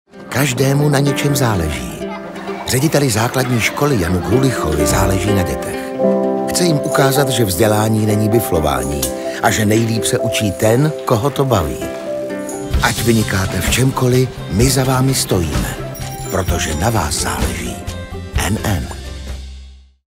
Jan Grulich - Obyčejní hrdinové (20´)
Nechte se inspirovat příběhy obyčejných hrdinů z různých regionů, kteří mají to štěstí, že díky svému odhodlání v životě dělají to, co je naplňuje.